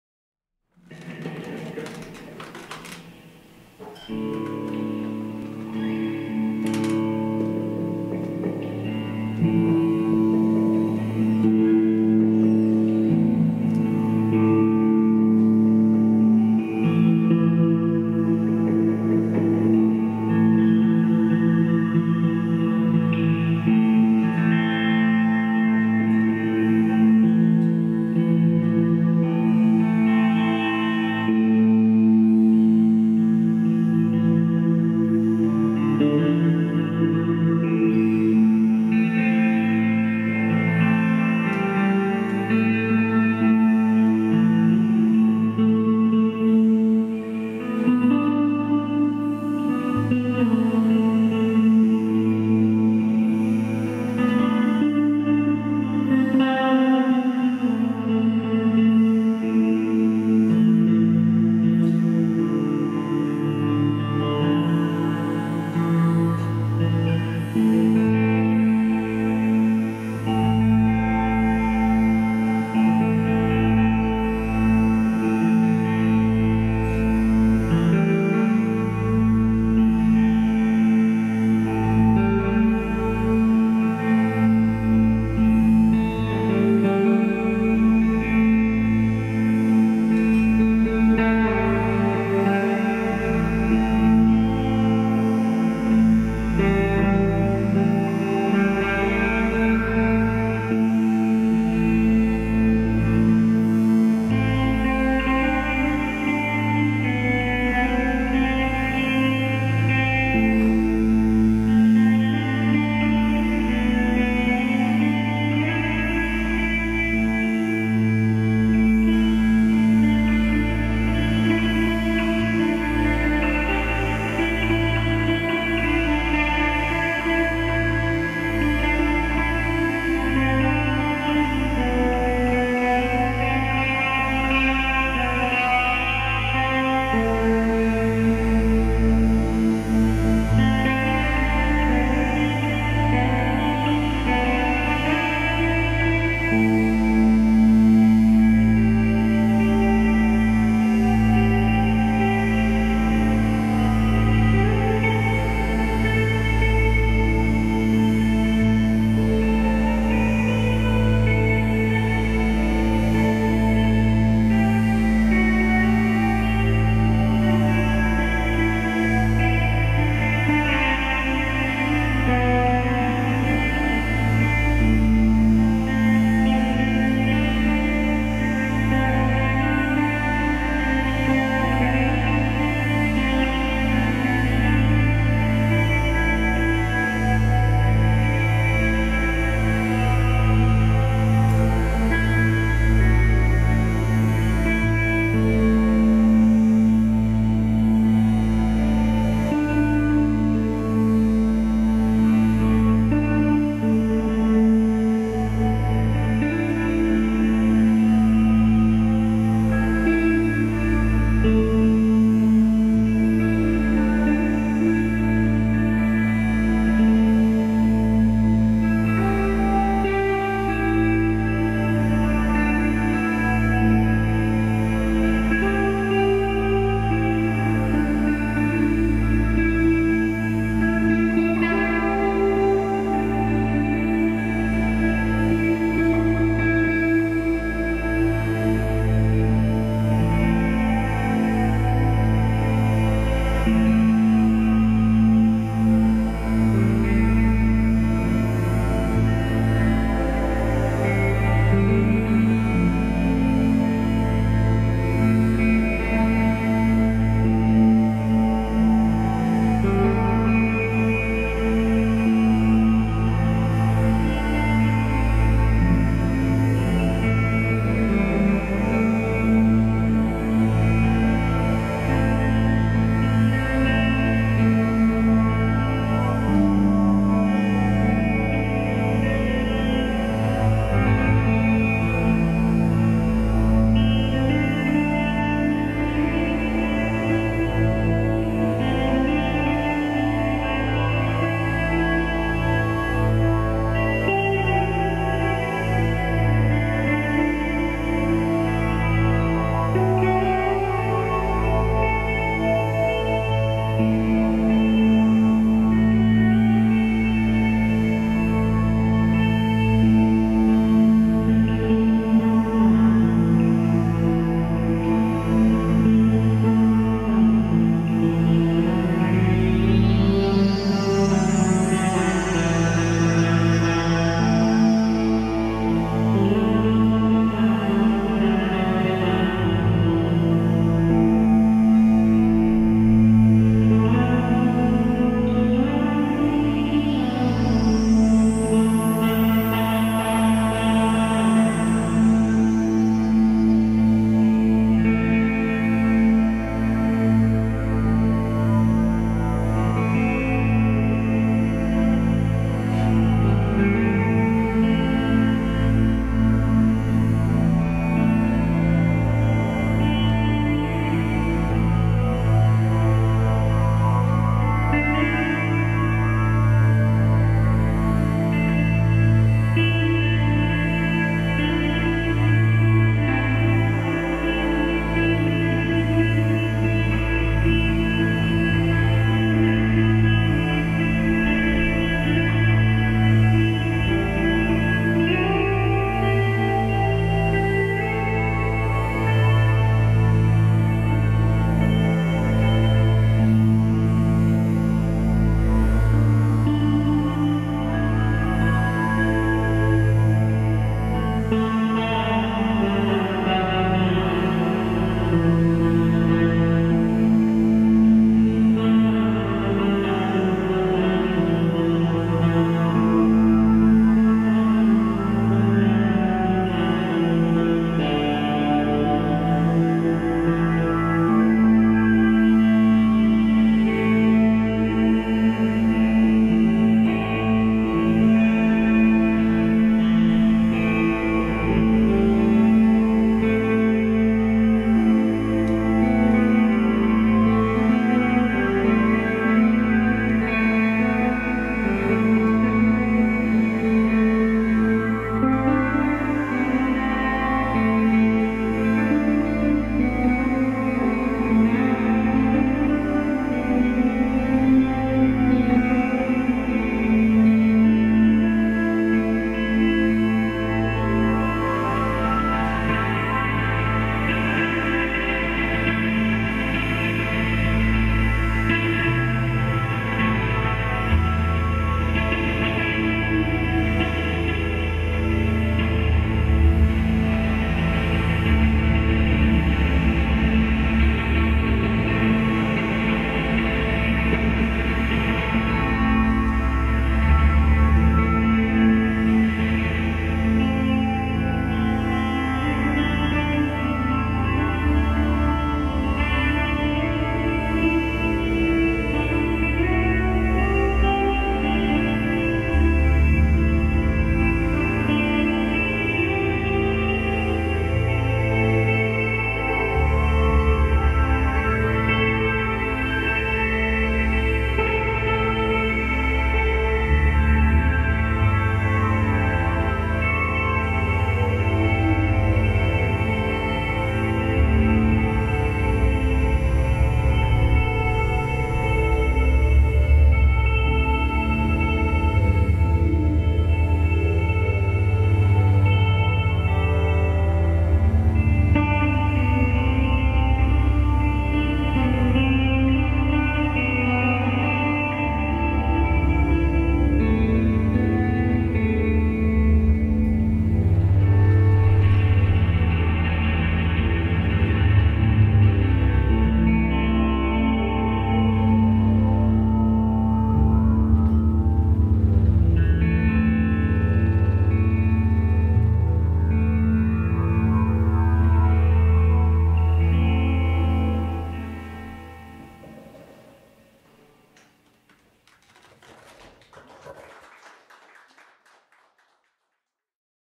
Howlarts 2015.02.28_3 Stefan Christoff & Catherine Debard
Howl! — A Sacred Cloud / Anarchist Mountains ep launch
Organisé par Howl Arts Collective et Jeunesse Cosmique
Samedi/Saturday 28 Février 2015, Sala Rossa

"join Jeunesse Cosmique / Howl Arts Collective for the launch of an ep featuring work by A Sacred Cloud + Anarchist Mountains

A Sacred Cloud.

C’est apprendre à connaître musicalement une autre personne, quelqu’un de complice. Une musique méditative, transcendante, de voyage à travers les sens. S’évader de son propre corps, esprit et de manœuvre. Être dans un état de purification, oublier les ennuies du monde moderne pour une lévitation pacifiste. Contrôler ses émotions au dépend de sa rationalité. Être libre de tout acte, de pensées et de négativités.

Stefan Christoff will play solo piano / guitar works, interspersed with street recordings, also joining the set will be Marco Simonsen-Sereda on saw / guitar.

P.W.Y.C. / Bring your own TEA."

Howl! arts collective